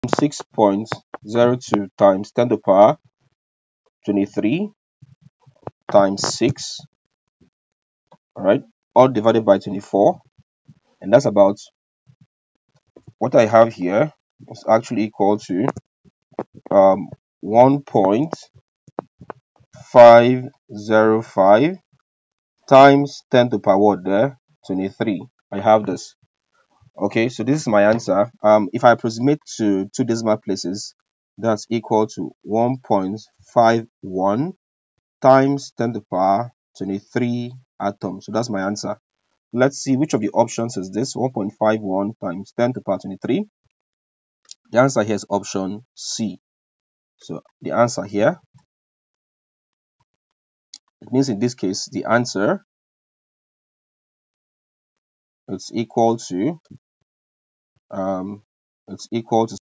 Mole Concept 3: Mole, Mass, Molar Mass, Avogadro's Constant and Number of Particles well explained.
This chemistry video teaches about the mole conept. In this video, you will learn what a mole is, as well as the mathematical relationship relating a mole to the mass, molar mass, number of particles and Avogadro's constant or number. If you want to lean all about the mole conept with examples, this video is for you.